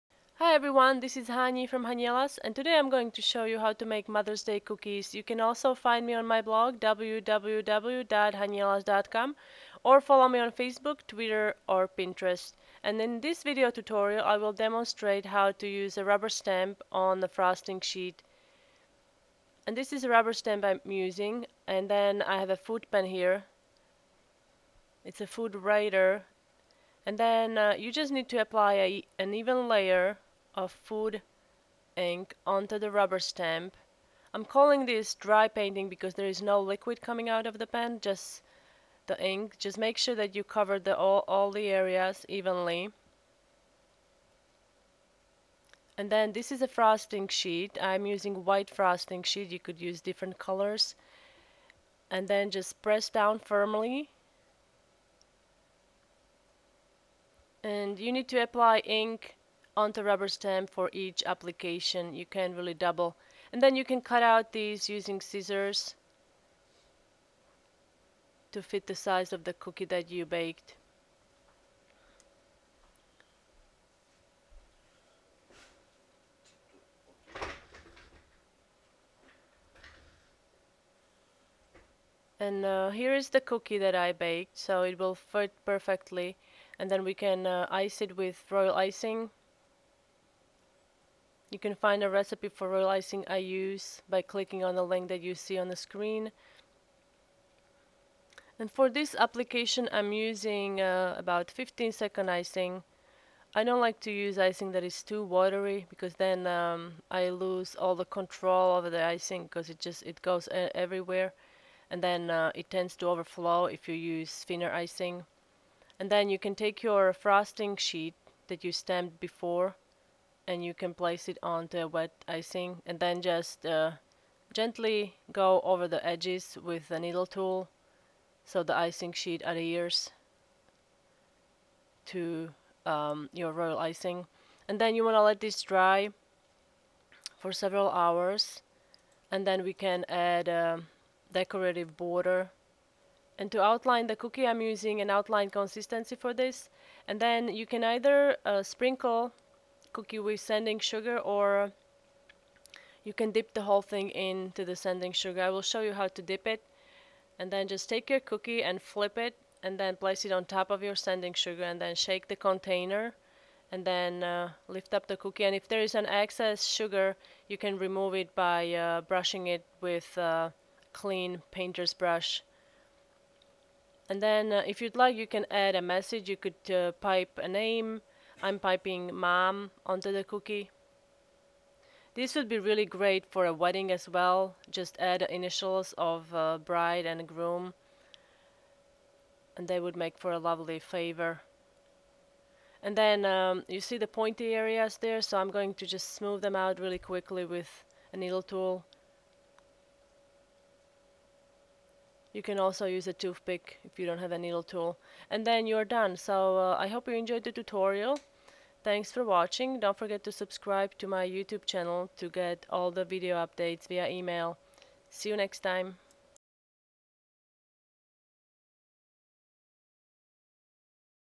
How To Decorate Cookies Using a Rubber Stamp on the Frosting Sheet, Mother's Day Cookies
In this video tutorial I demonstrate how to use a rubber stamp on the frosting sheet making pretty Mother's Day Cookies. Adding different personalized message these could be also used for a wedding, engagement party or Valentine's day.

DON'T BE A STRANGER, SAY HELLO.